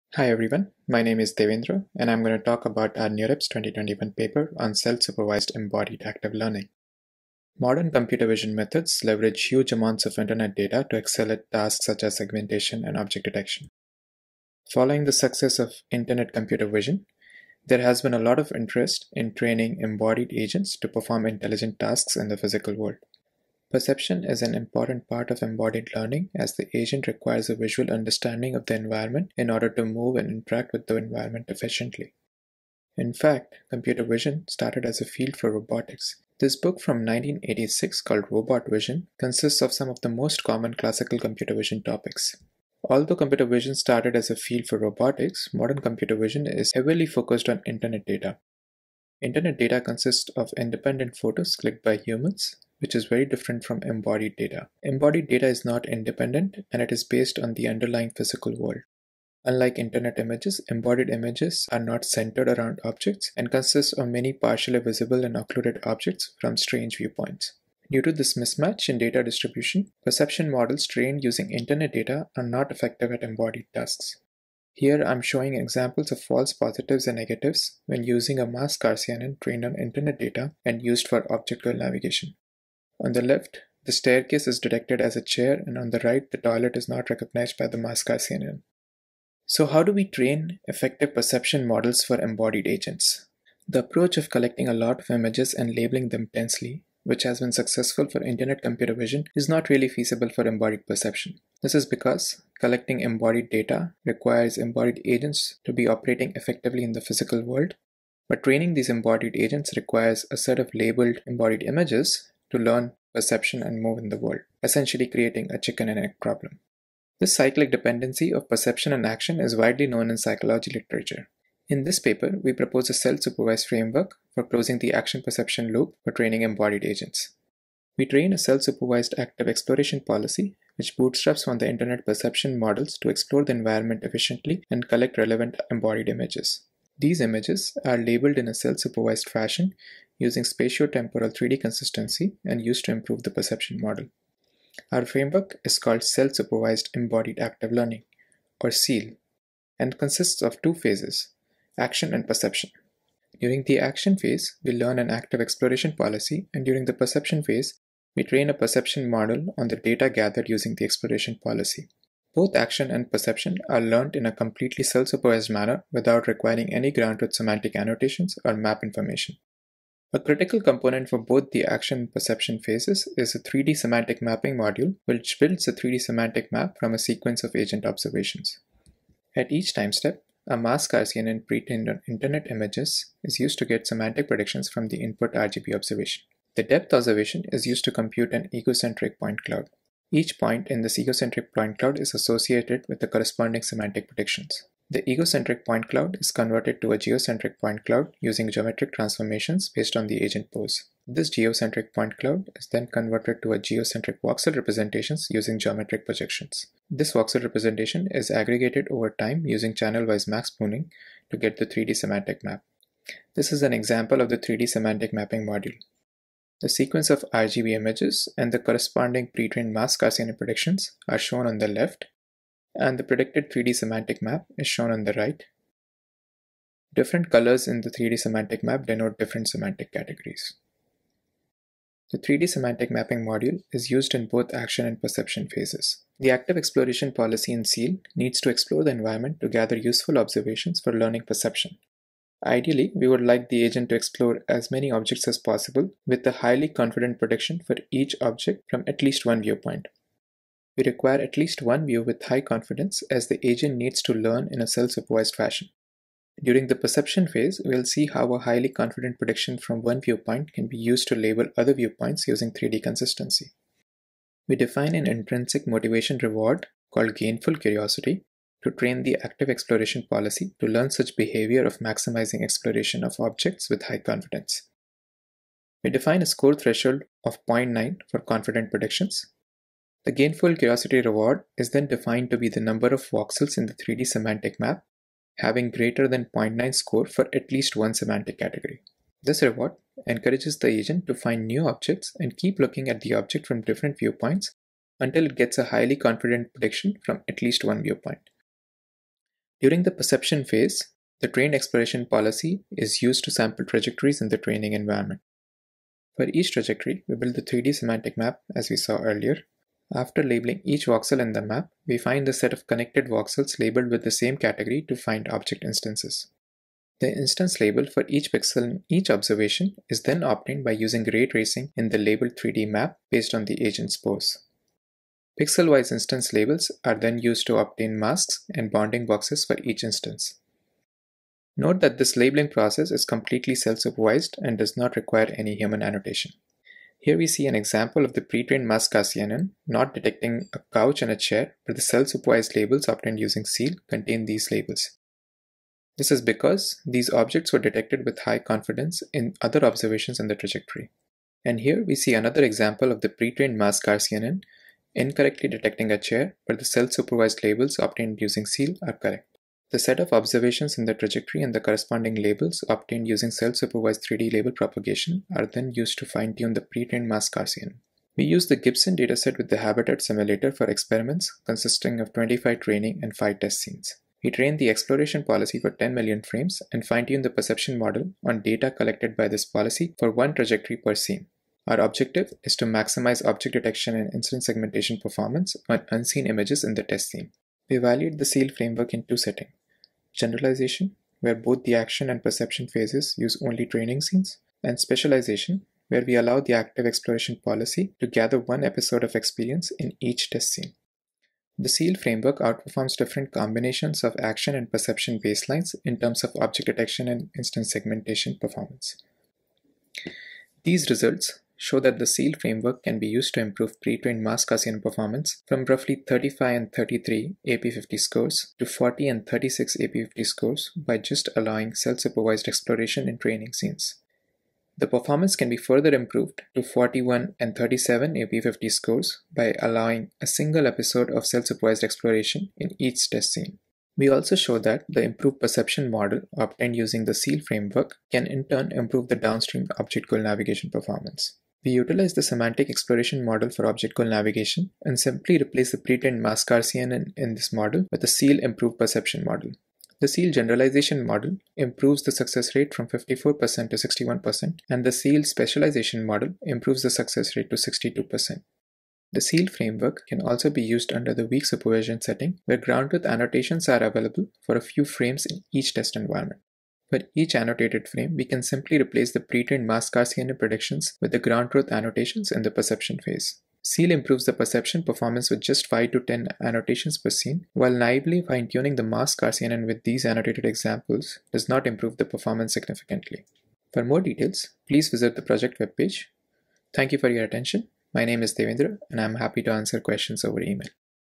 SEAL: Self-supervised Embodied Active Learning (NeurIPS 2021)
Presentation for the NeurIPS-2021 paper, "SEAL: Self-supervised Embodied Active Learning".